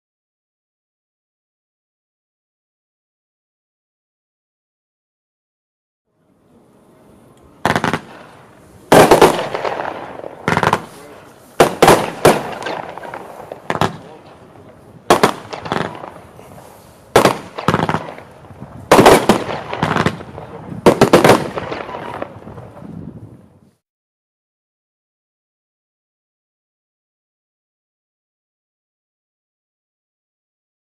Xplode No Fear Batteriefeuerwerk Silvesterfeuerwerk
Feuerwerksbatterie No Fear von Xplode Feuerwerk mit gefächerten Gold-Effekten.

Xplode No Fear direkt online kaufen im Feuerwerk Onlineshop von Röder Feuerwerk

Original Verkaufsware, gezündet beim großen Testschießen von Röder Feuerwerk am 09. April 2016 in Schlüsselfeld.

Aufgenommen haben wir dieses Feuerwerk-Video wie immer mit unserer Panasonic GH4 - auch in 4k verfügbar!